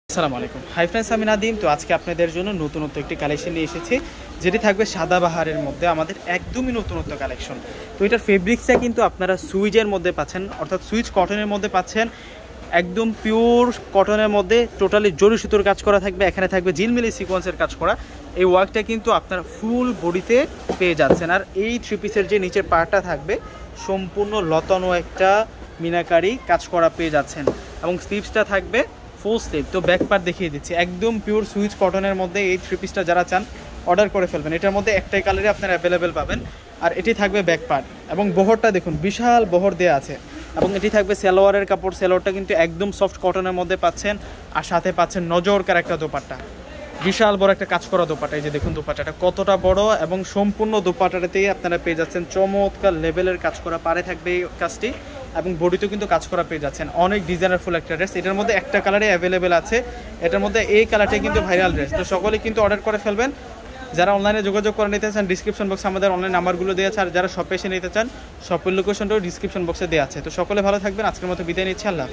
পূর্ণিমা শাড়িতে ৮০০ টাকায় সাদাবাহারের ভাইরাল থ্রি-পিস এর মেলা বসেছে
Purnima saree